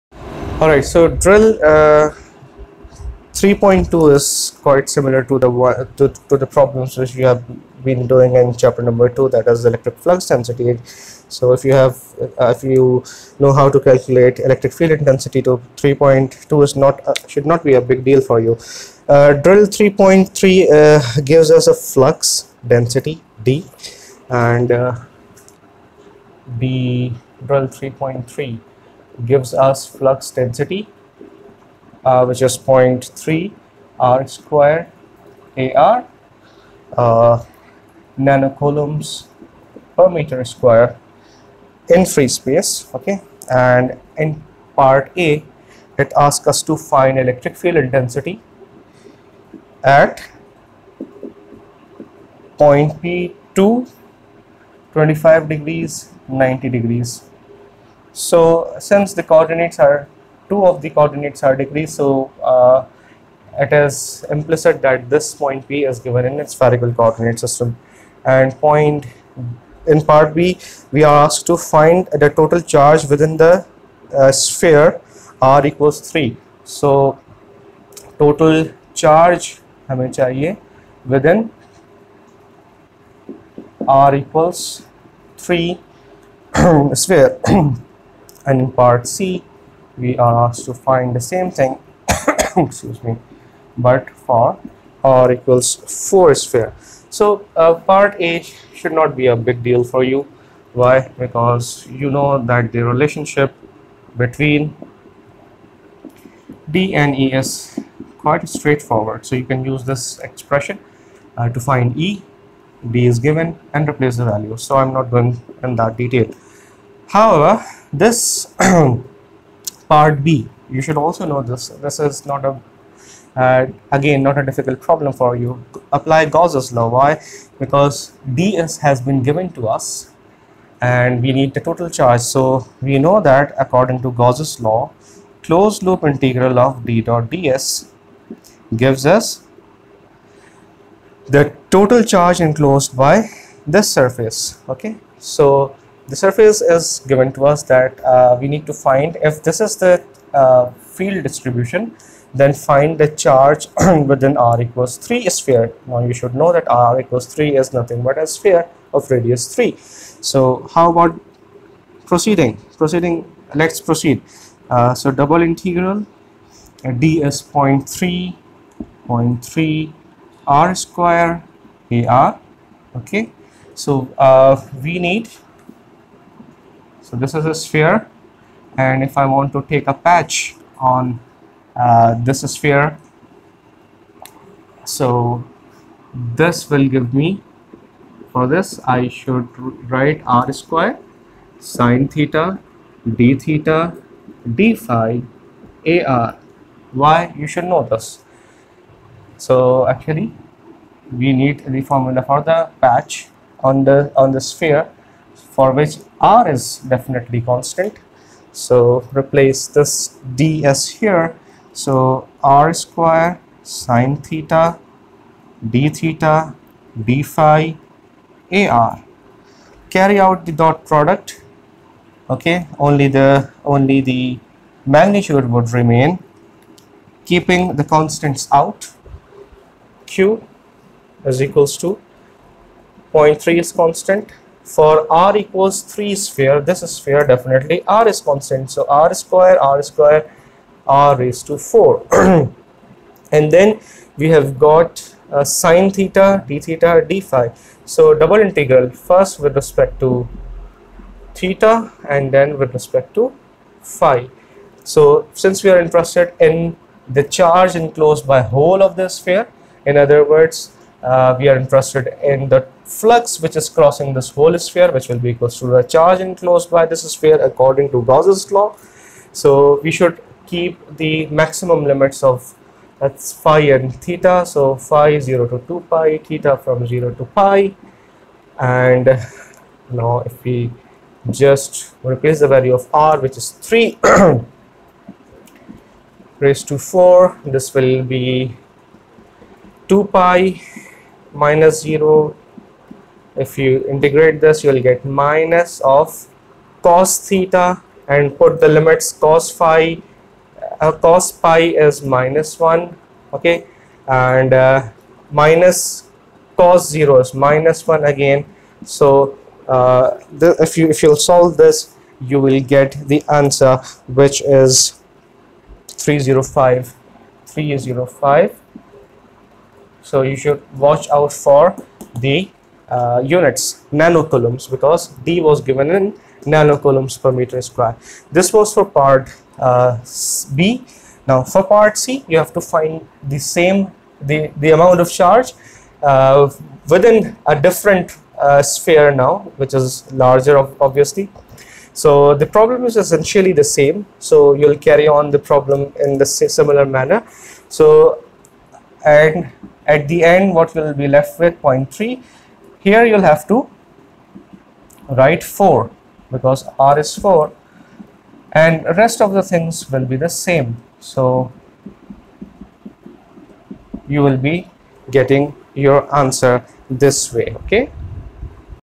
Drill Problem 3.3
Drill problems of William Hayt (8th Edition).

Chapter 3: Electric Flux Density, Gauss's Law, and Divergence.

Recommended Playback Speed: 1.5x